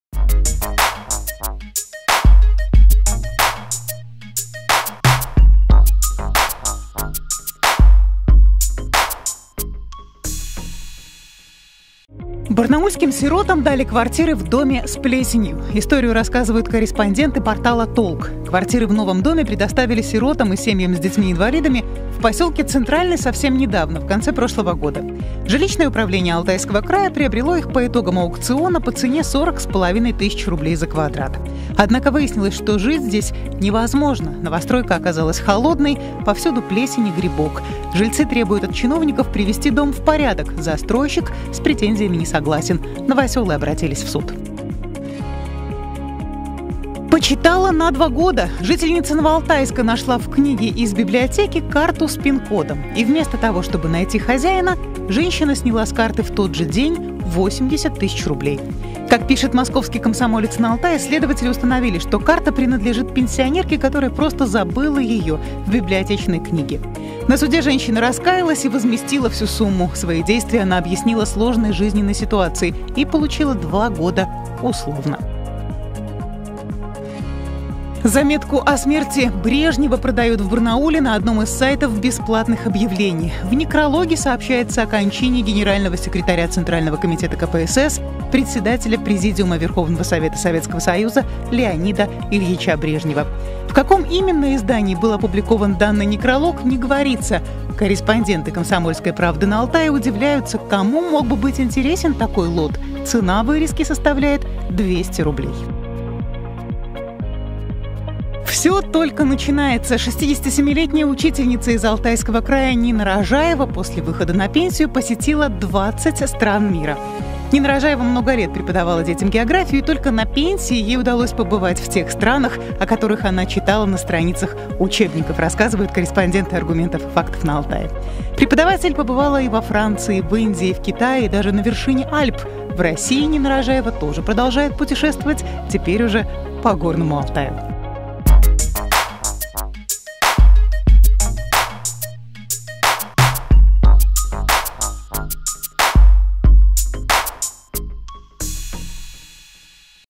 О чем пишут издания «Сибирской медиагруппы» — обзор 19.02.2019
Сколько стоит газетная вырезка с сообщением  о смерти Брежнева,  и как сделать пенсию нескучной – в традиционном обзоре публикаций журналистов Сибирской Медиагруппы.